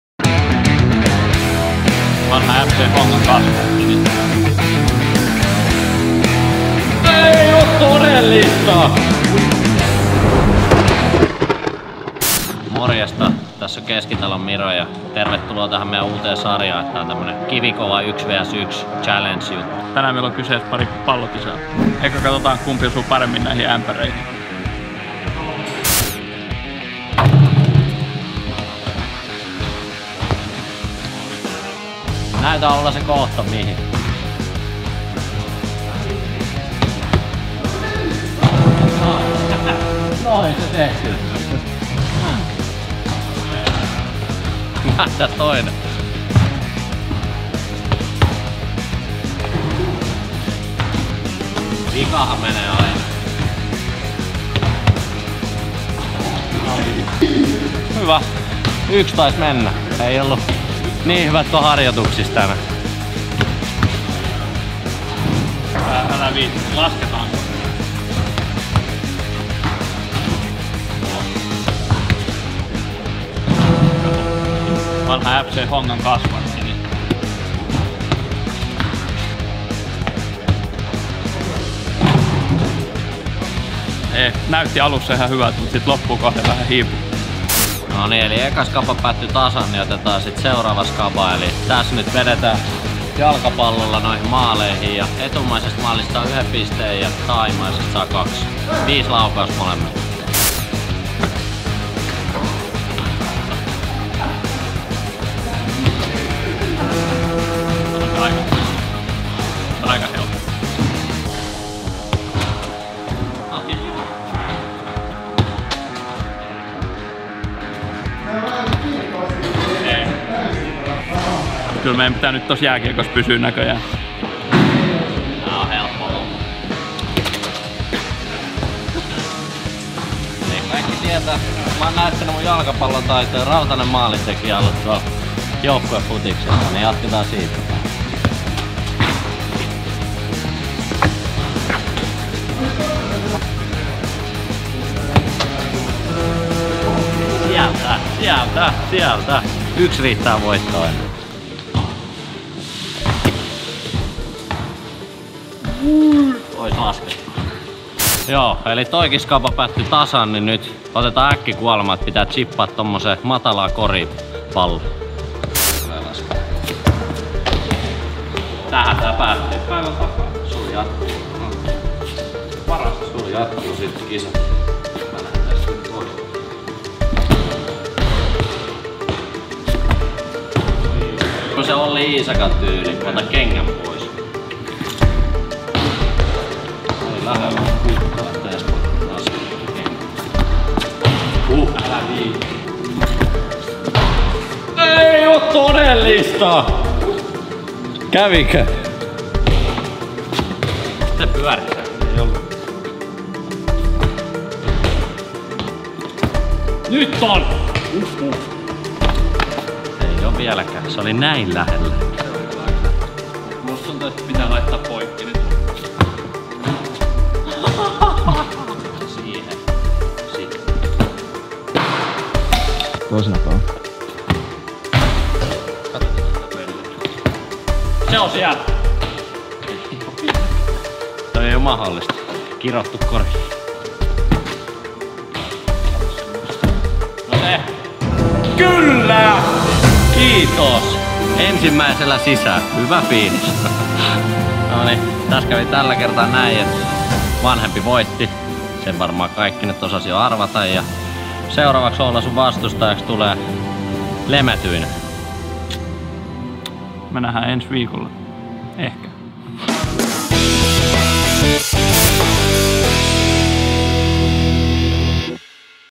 KiviKova 1 vs. 1 - Miro vs. Oula
Uudessa videosarjassamme taistellaan mitä kummallisempien haasteiden parissa. Ensimmäisessä jaksossa vastakkain ovat Miro Keskitalo ja Oula Kuure. Voittaja pääsee irti mediavastuista ja häviäjä jatkaa seuraavaan jaksoon.